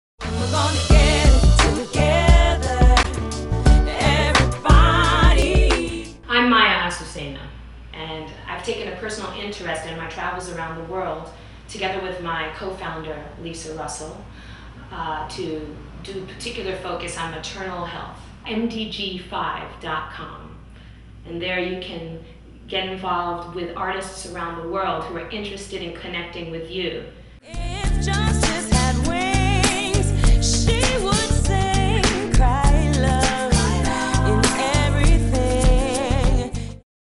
Maya Azucena on MDG 5
Maya Azucena speaks on Millennium Development Goal 5 (Maternal Health) for the International Day of Peace Global Broadcast.

Brooklyn singer/songwriter Maya Azucena believes that music is a tool for healing and activism, and supports humanitarian causes with her voice.

Maya was commissioned to write two original songs for IFC documentary Lockdown USA; has contributed music to Emmy-winning UN documentarian Lisa Russell's latest film "Not Yet Rain," regarding the advancement of Women's Health laws and reproductive rights in Ethiopia; was invited two years in a row to be the official post-race entertainment at the Susan G Komen Race for the Cure in NYC's Central Park; and produced Hope Night, a concert night of domestic abuse awareness which brought together four nonprofit organizations.

Recently a Grammy winner for her duet on Stephen Marley's "Reggae Album of the Year," named Best Alternative Artist by AllHipHop.com, winner for three awards including Best Female Vocal in the international competition for ABC Radio Network's Fame Games EFFIGY Awards, and two-time nominee for Best Female Vocalist of the Year at the Reader's Choice Soul Tracks Awards